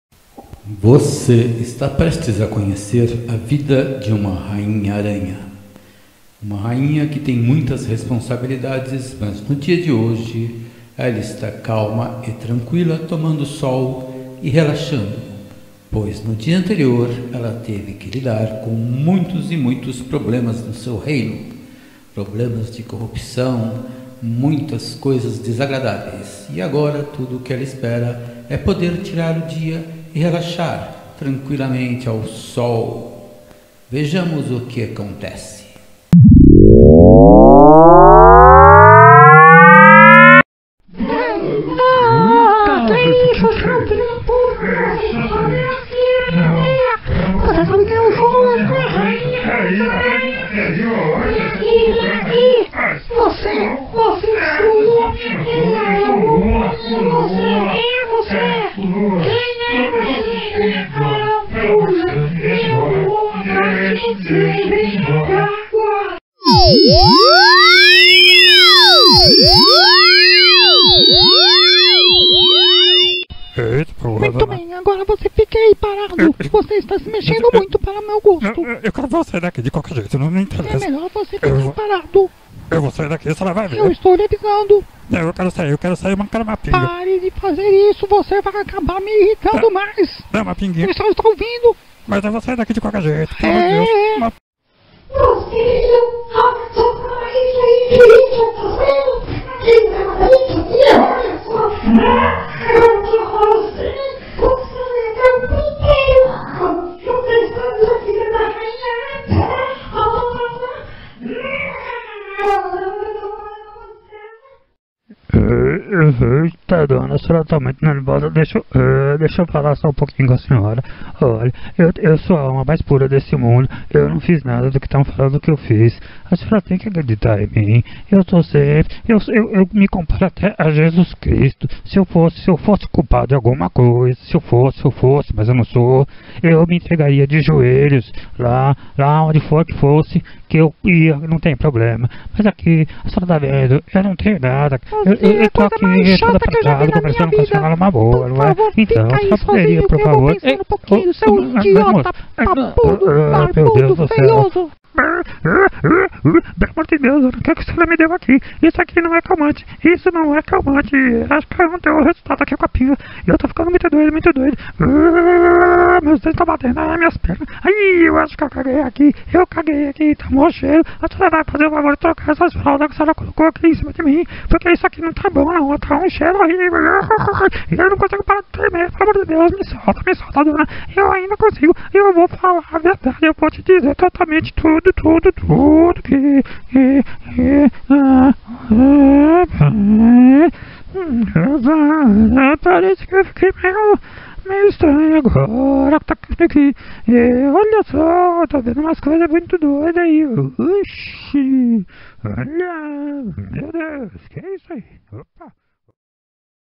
The Queen Spider
The Movie.
With Monica Bellucci as The Queen.
Well, I did her voice. She did not come. Why???
So, no Monica Bellucci.
Voices, movie, light, audio, direction, production. ALL, I have to do.

I guess that the dialogs make sense only in Brazil. Sorry guys.